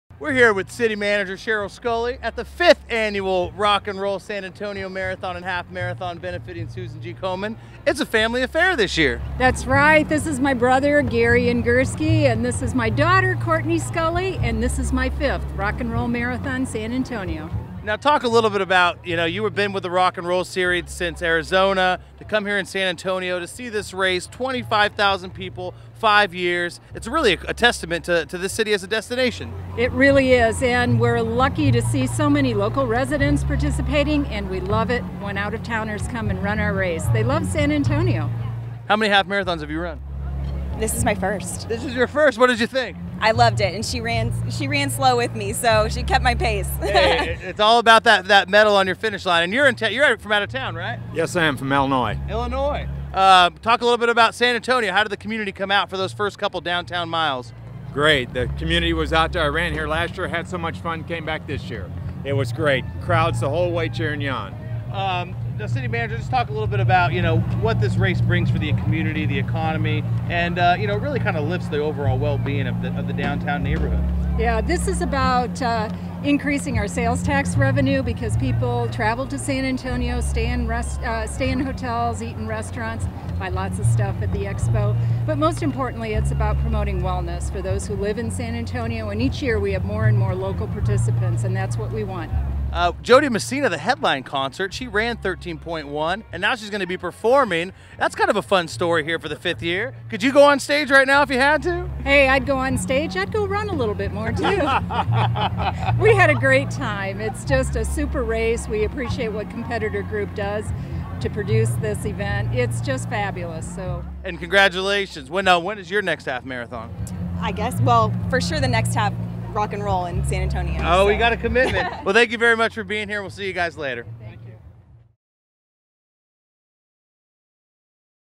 2012 Rock 'n' Roll San Antonio Sheryl Sculley Interview
Listen to what Sheryl Sculley, city manager, has to say about running the San Antonio marathon with her brother and daughter.